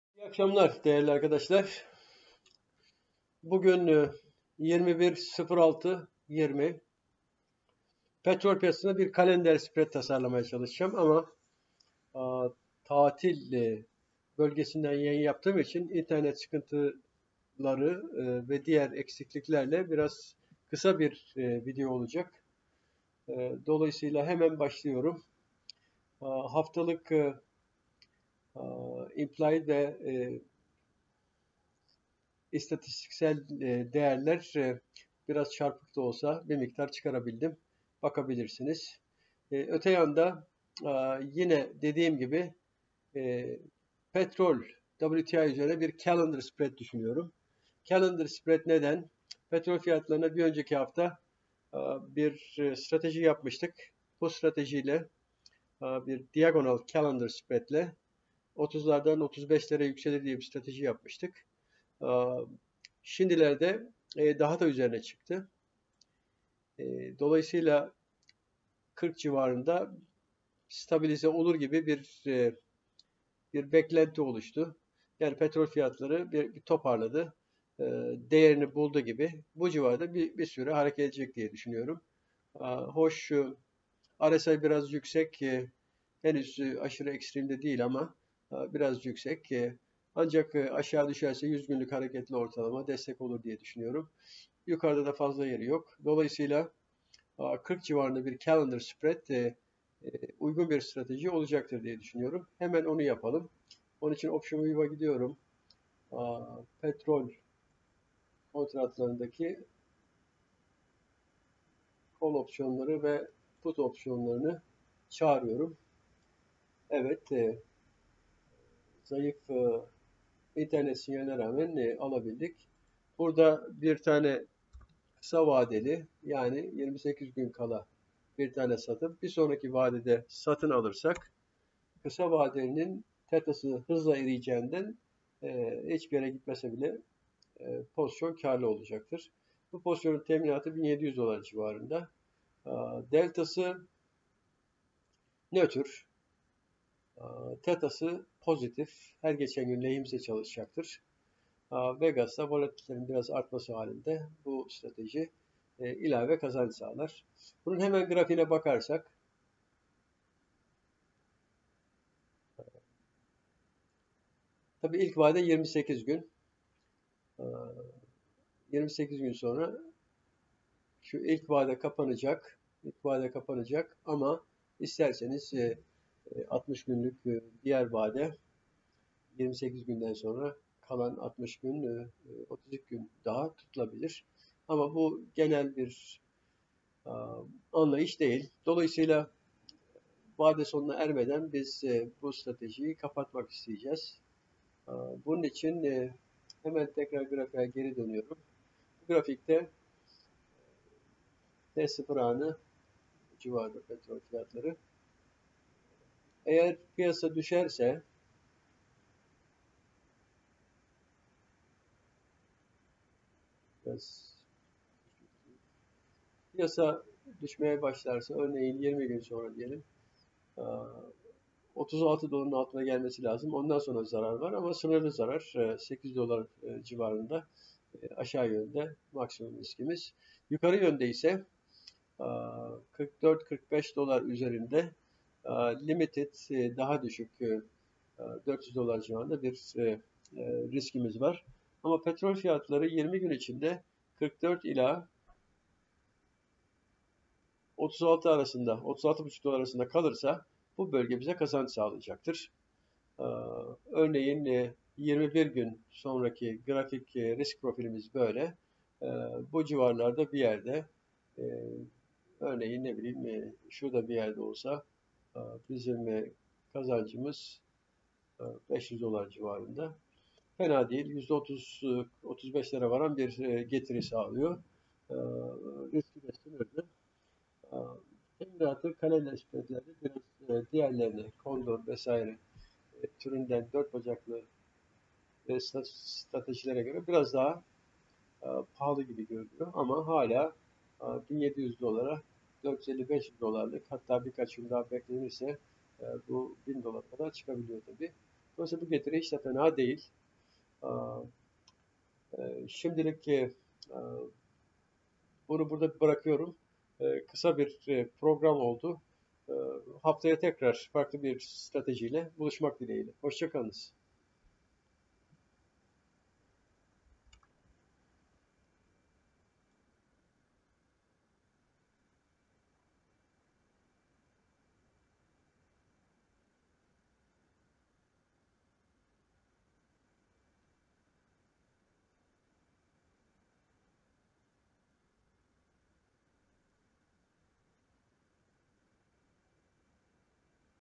21 06 20 Opsiyon Raporu WTI Petrol için Calendar Call Spread tasarımı
riski sınırlı yüksek getirili strateji